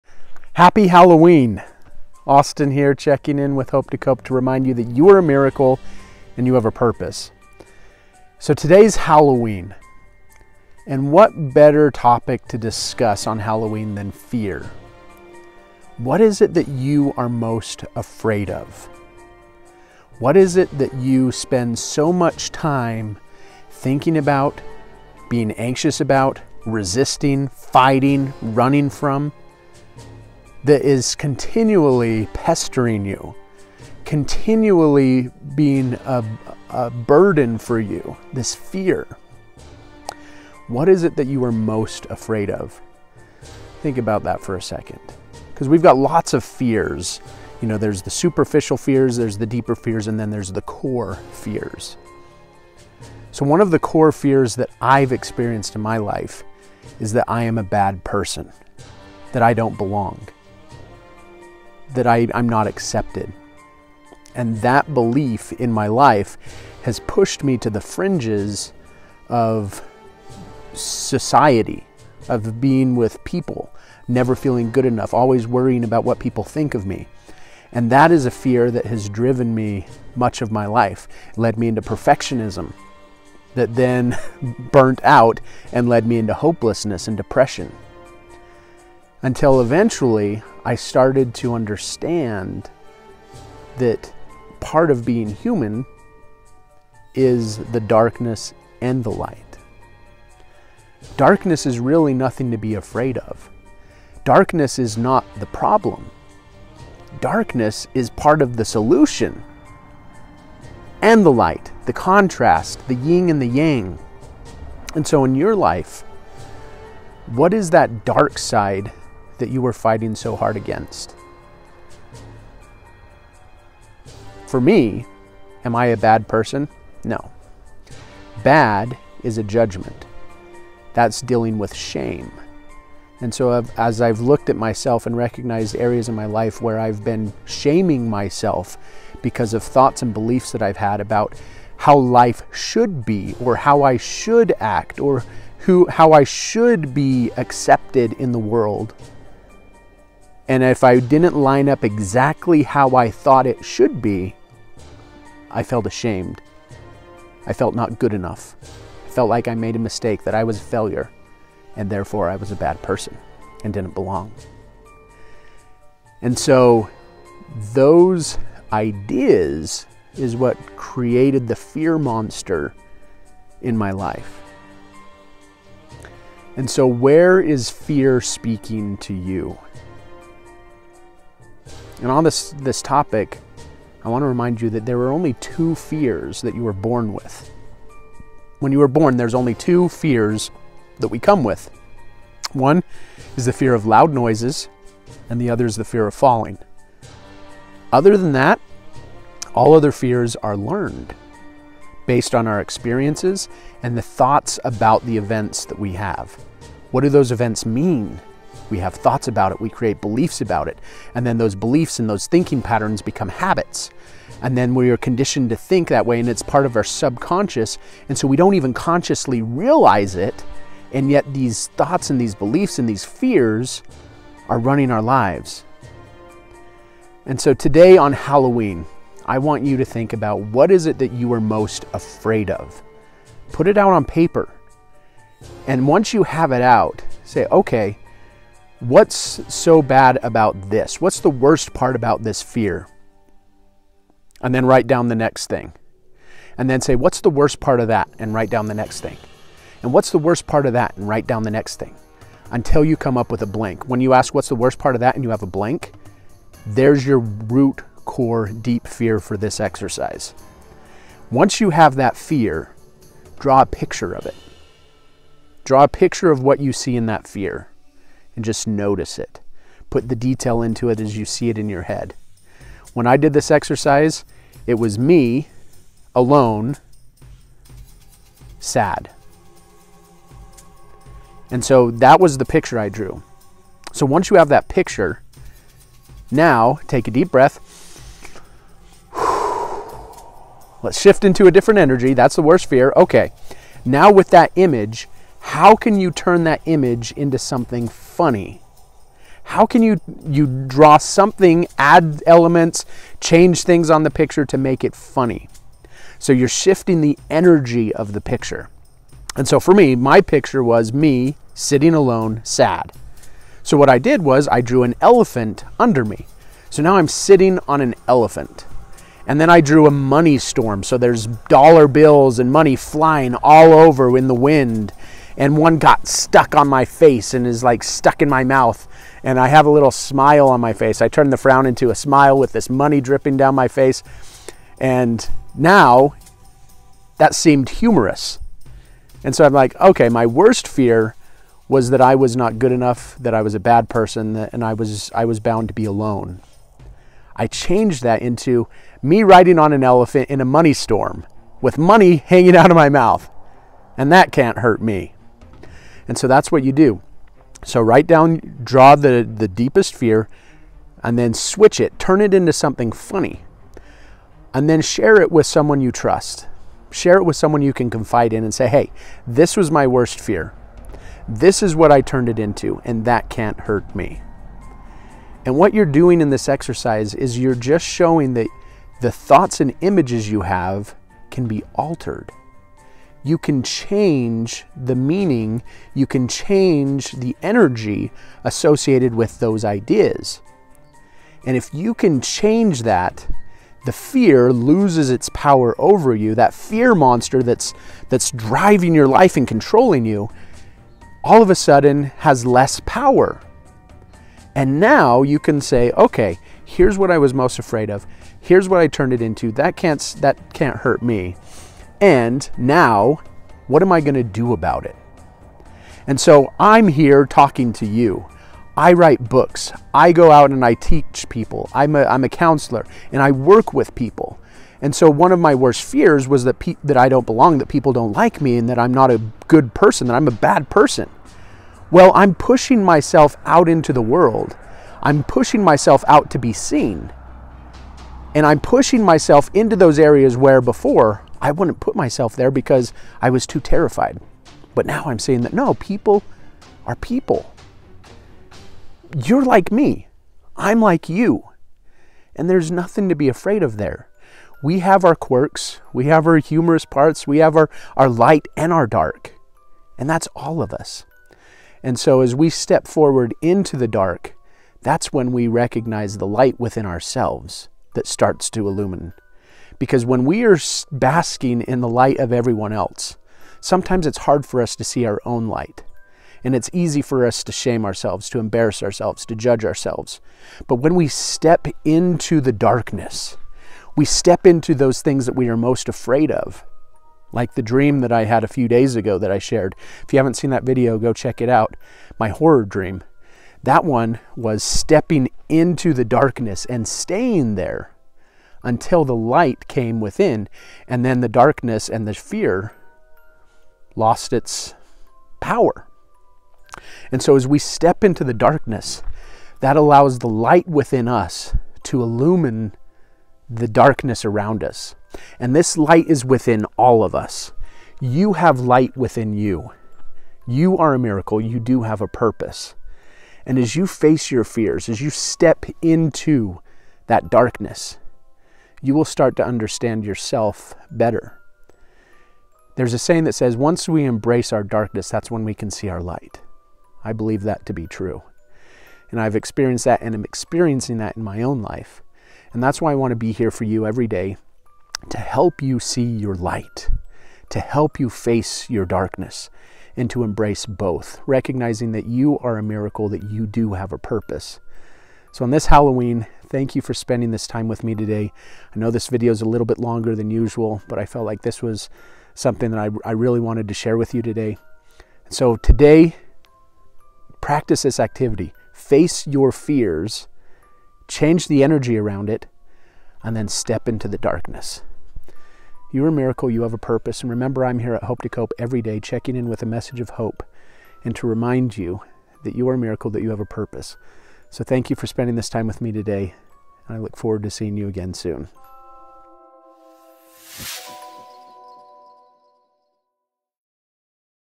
What are you most afraid of?
A reminder about fear and it’s significance in our lives. You are not powerless to fear. You can face your fears and move through them. In this video I share a simple activity you can do with one of your fears. Try it out and let me know how it goes. You are a Miracle! You have a Purpose!